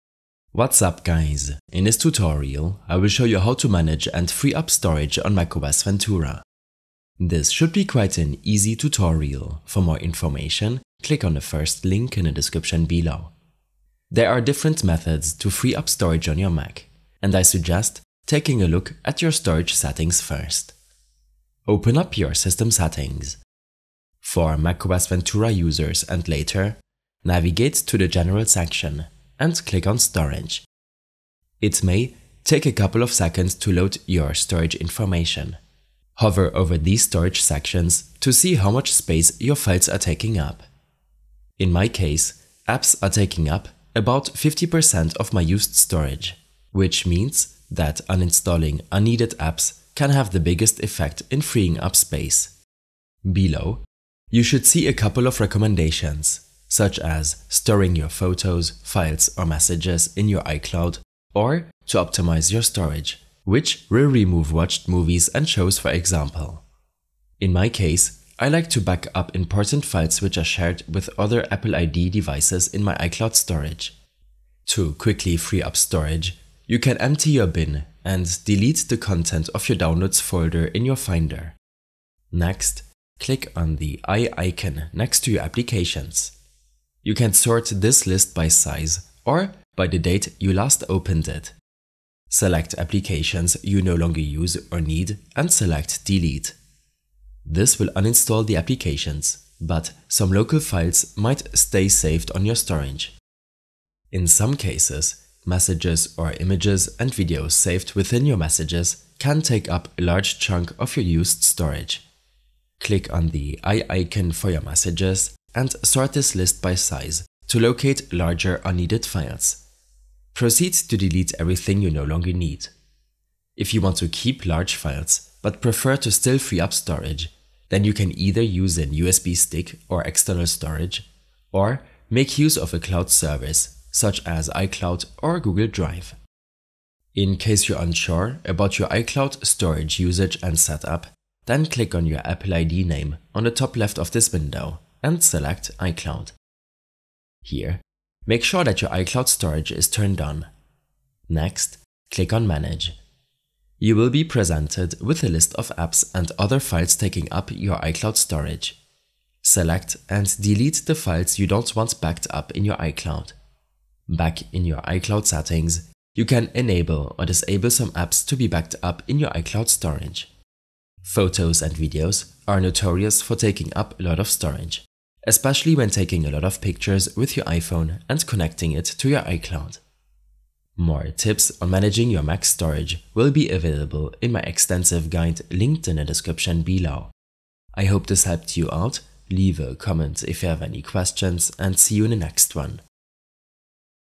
How To Manage & Free Up Storage on Mac OS Ventura
A short tutorial on how to manage and free up storage on macOS Ventura.

Timestamps:
0:00 Introduction
0:13 Storage Settings
0:53 Storage Recommendations
1:14 Free Up Storage
1:26 Uninstall Applications
1:44 Delete Messages
2:03 iCloud Settings
2:30 Manage iCloud Storage